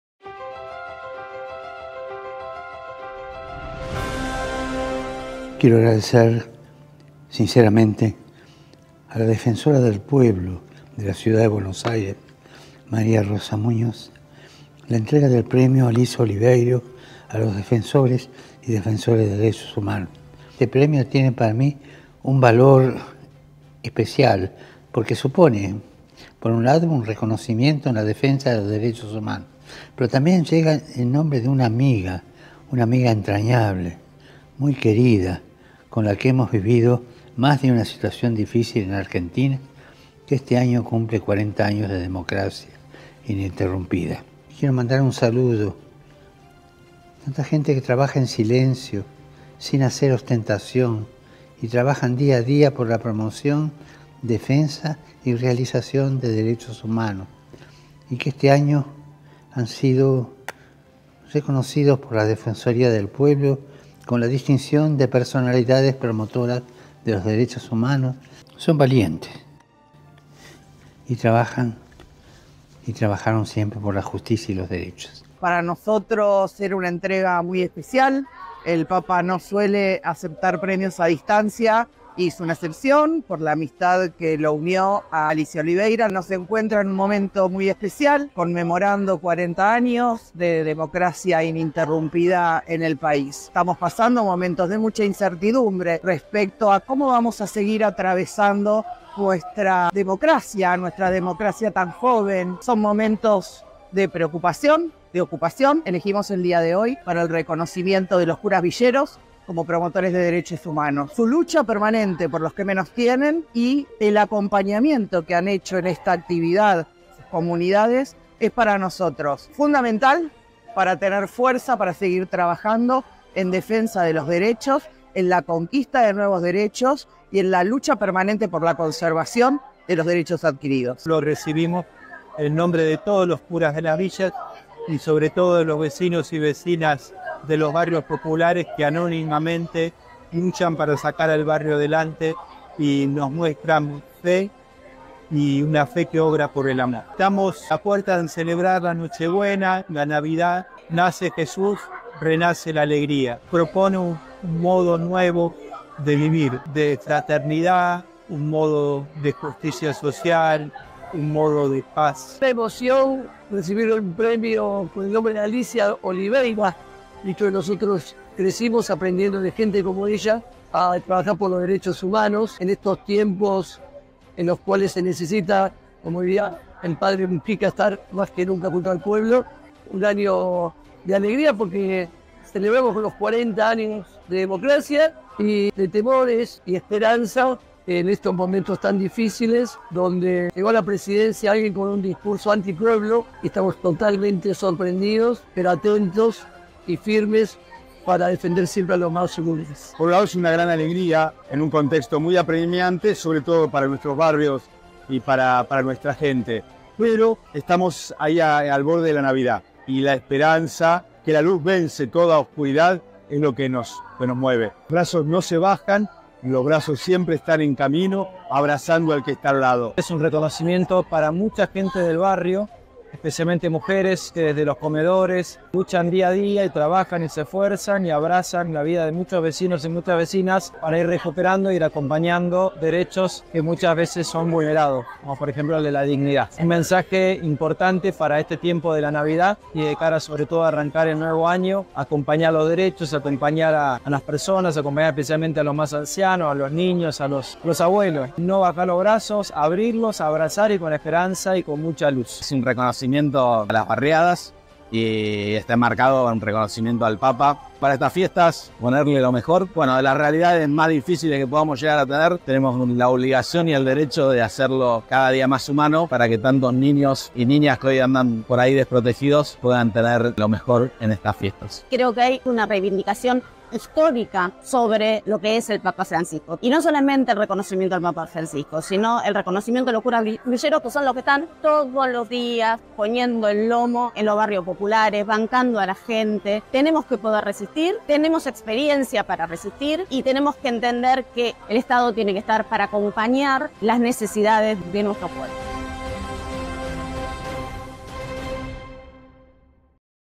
El Papa Francisco recibió el premio Alicia Oliveira 2023
En el marco del cuadragésimo aniversario del retorno democrático en Argentina, el Sumo Pontífice aceptó recibir esta distinción destinada para quienes contribuyen desde su tarea en la defensa de los derechos humanos.